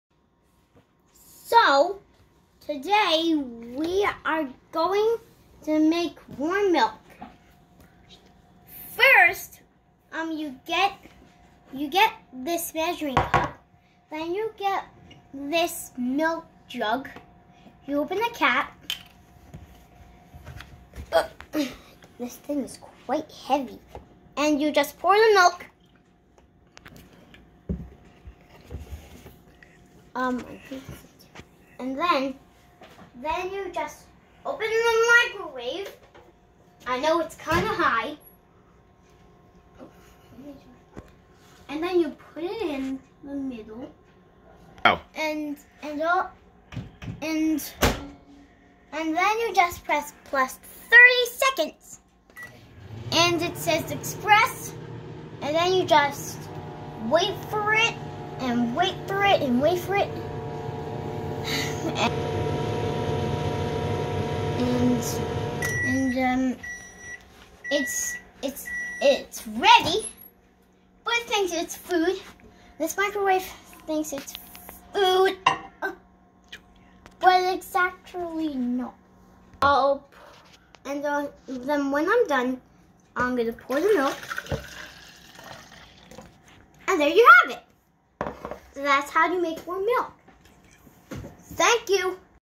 (MOST VIEWED!!!) How to make warm milk in the microwave
Michael shows his friends how to make warm milk carefully using the microwave.